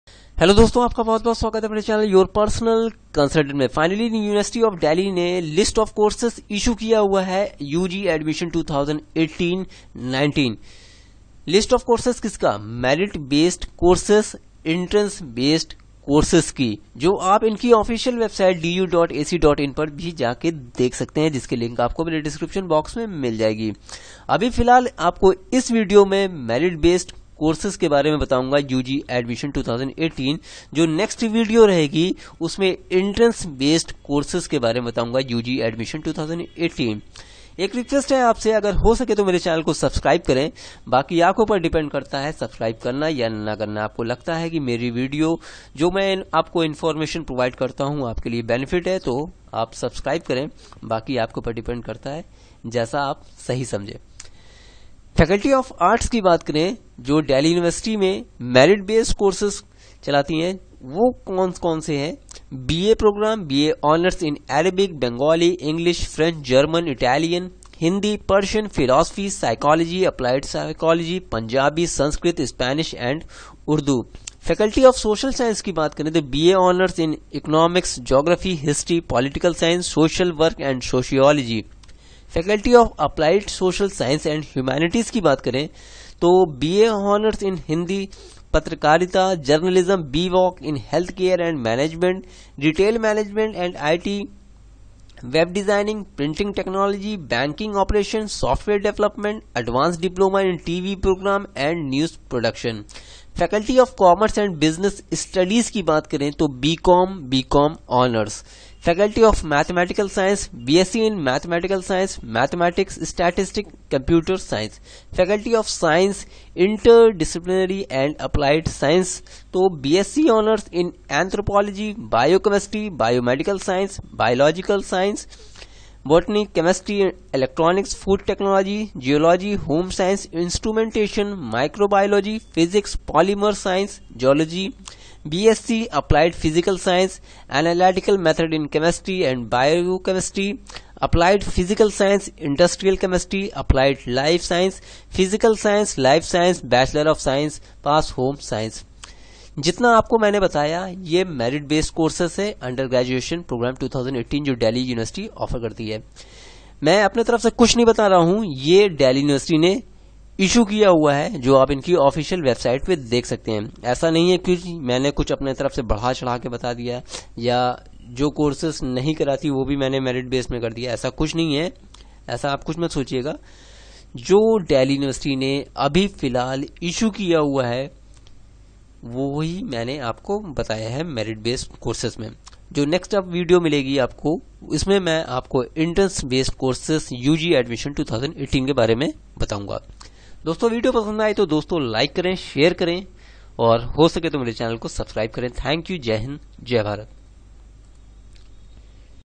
Delhi University 2018 Admission procedure | New changes in DU Admissions | DU UG merit based courses
UR PERSONAL CONSULTANT
PERSONAL CONSULTANT